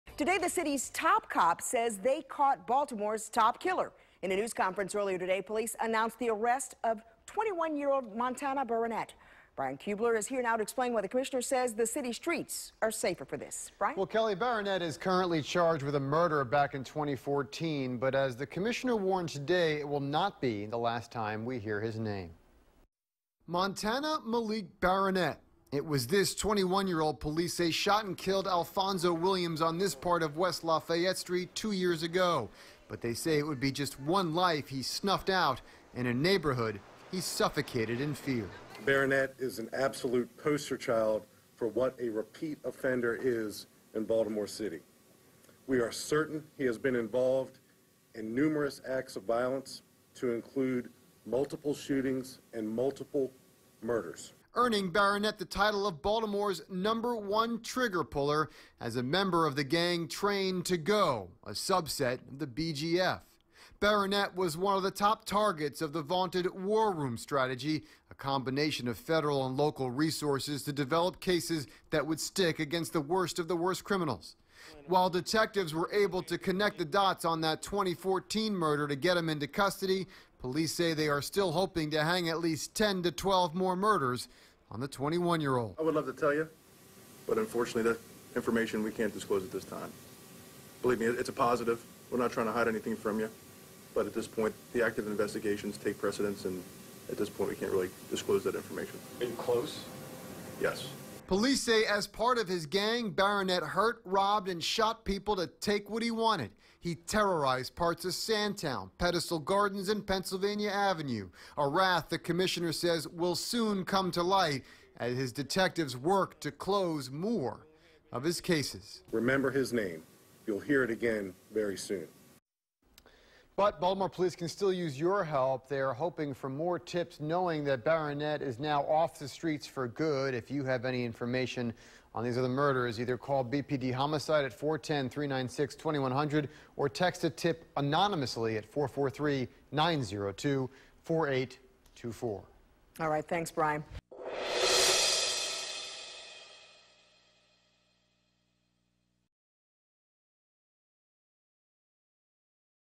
Baltimore Police arrest man they call city's "No. 1 trigger puller"
A man police refer to as Baltimore's top trigger puller has been arrested and charged with murder.